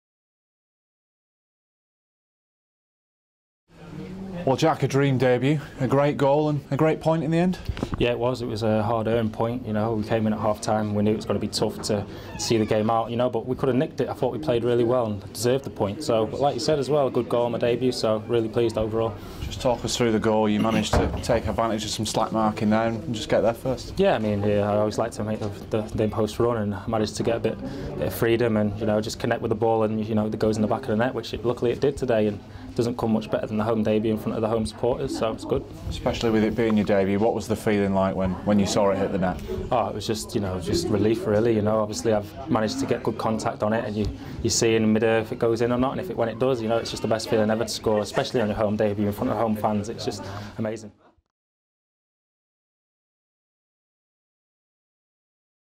Jack Rodwell reflects on home debut against Manchester United
Post match interview with Jack Rodwell after he scored on his home debut against Manchester United.